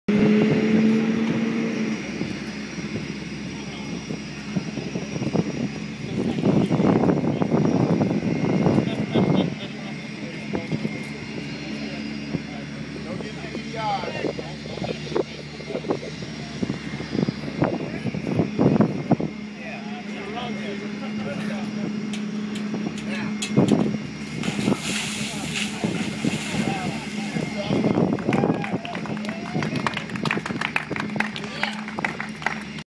Last Flight Of Our Current Commanding Officer
The return party to the last flight of our current CO.